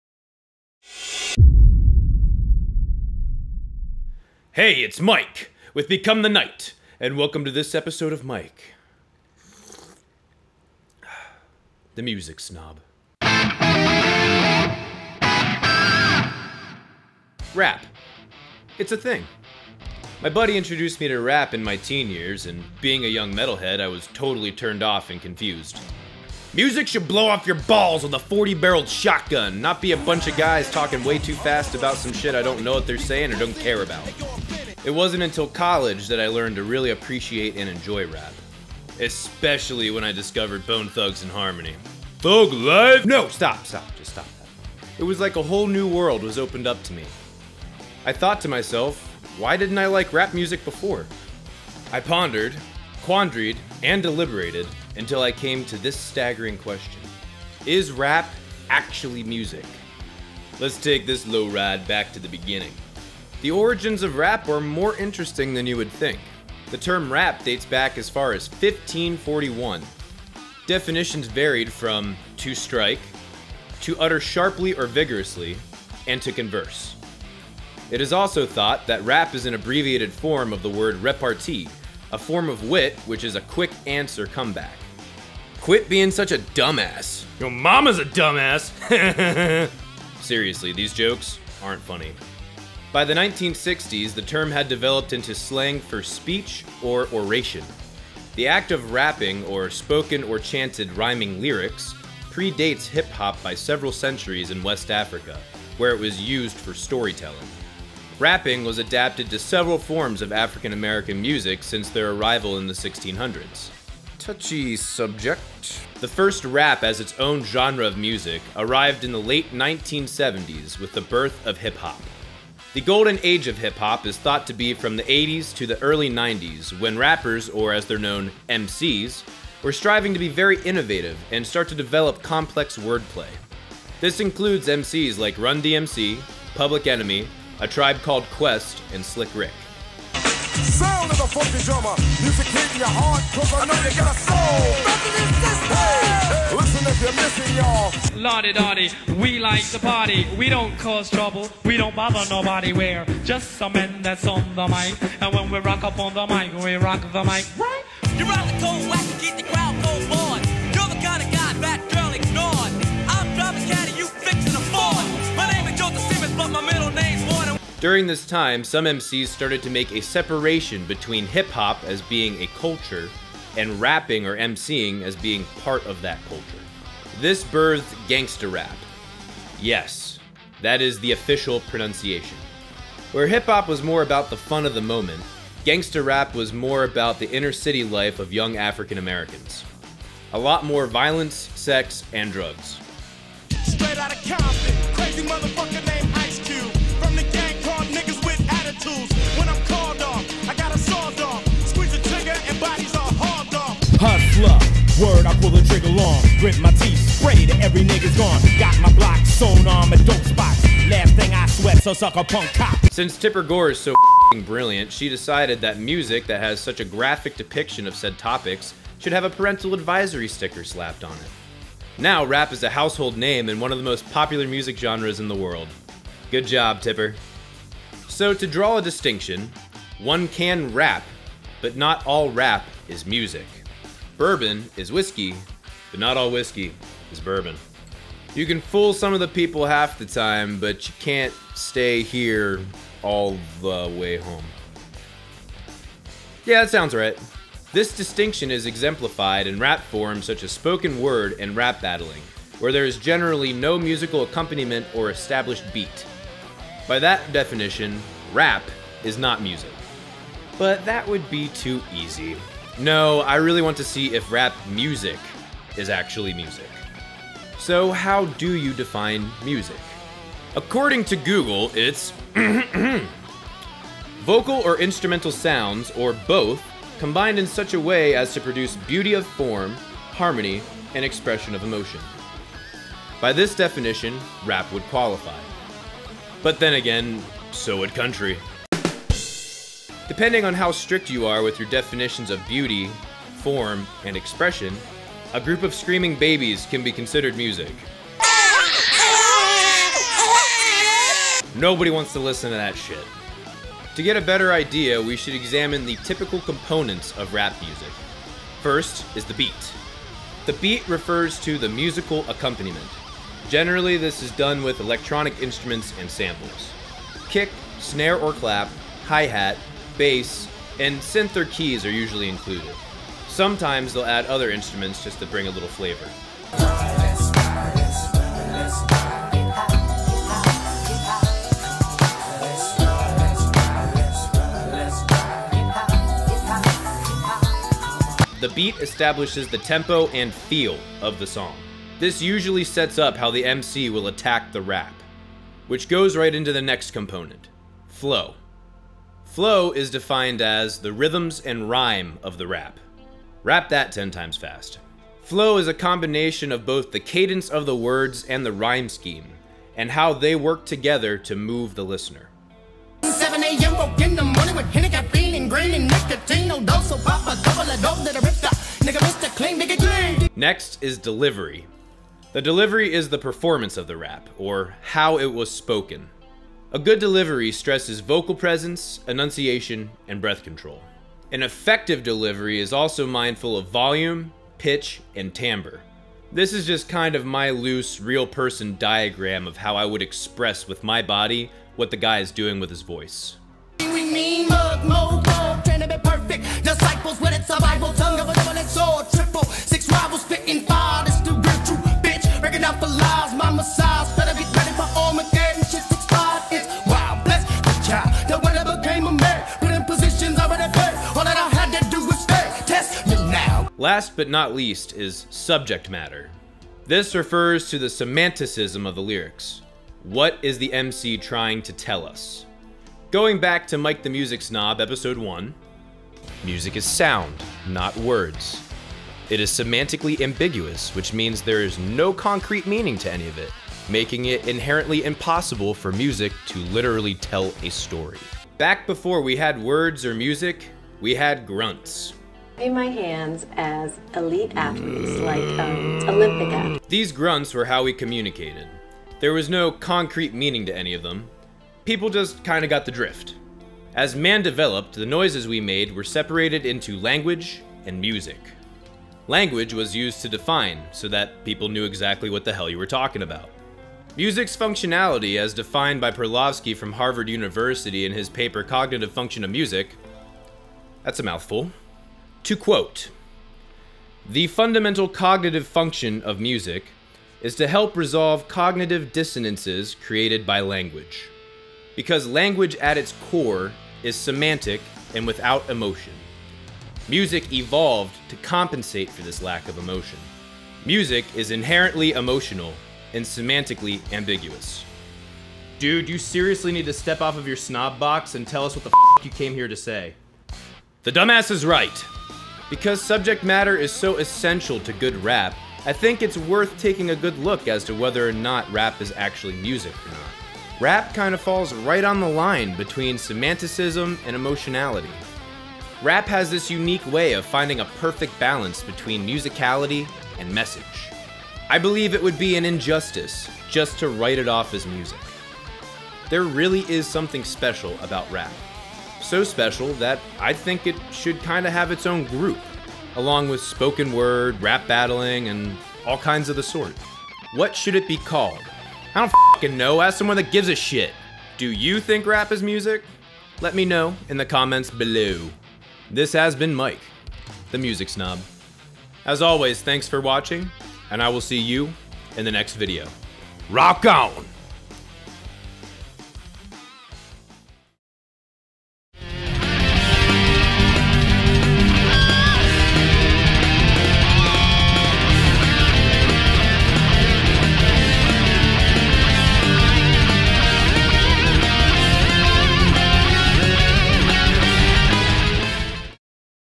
IS RAP REALLY MUSIC? | Mike The Music Snob Ep 5
Is rap music ACTUALLY music? Watch to find out.